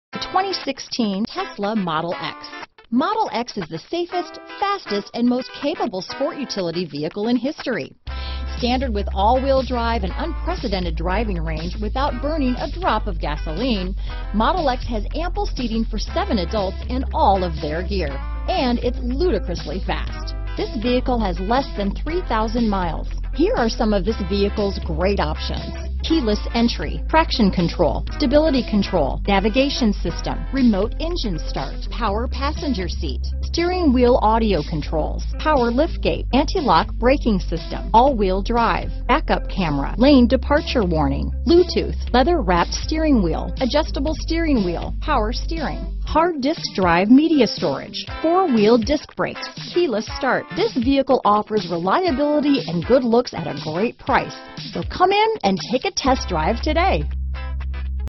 2016 Tesla Model X Atlanta, Marietta, Decatur, Johns Creek, Alpharetta, GA 7284A
Obsidian Black Metallic Used 2016 Tesla Model X available in Atlanta, Georgia at Hennessy Ford. Servicing the Marietta, Decatur, Johns Creek, Alpharetta, GA area.
2016 Tesla Model X 75D - Stock#: 7284A - VIN#: 5YJXCBE20GF027920

Hennessy Ford
5675 Peachtree Industrial Blvd